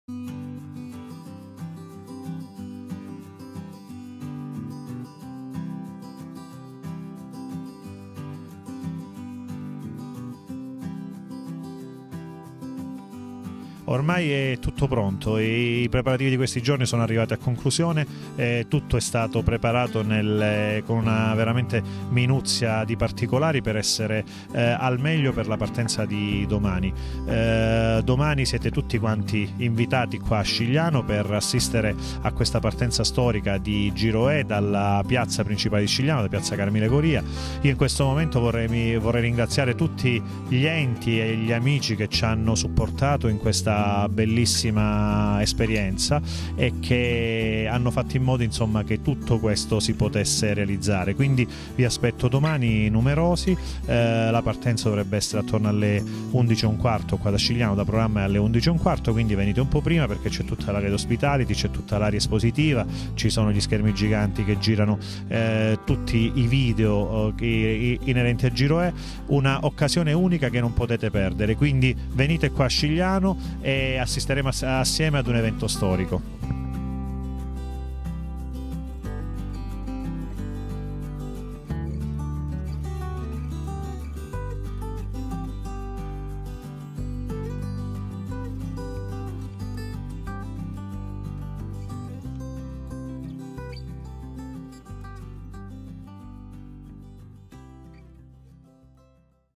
Intervista al Sindaco Raffaele Pane - Partenza da Scigliano - Giro E 2020
Partenza GiroE da Scigliano (Cs) - Intervista al Sindaco Raffaele Pane. A cura di Antonio Abbruzzese
Tappa 4 del 7 ottobre 2020:  Scigliano - Camigliatello Silano